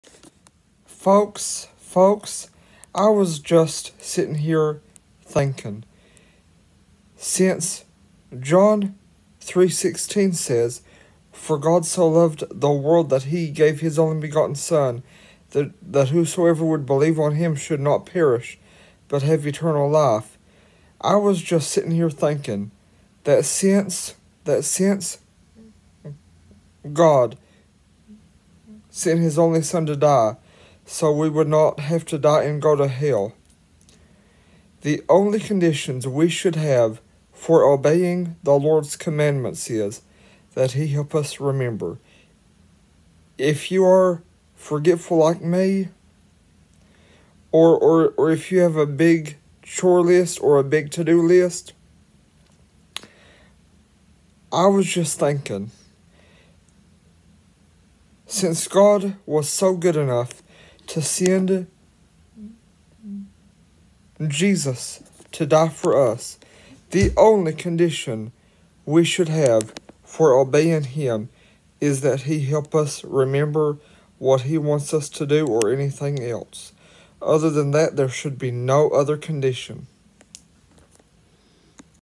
WordFrom God obedience tipAdvice to help us￼￼Obey God more￼
Word From God the purpose of this video is to help us obey God more with a great start suggestion and idea that I had￼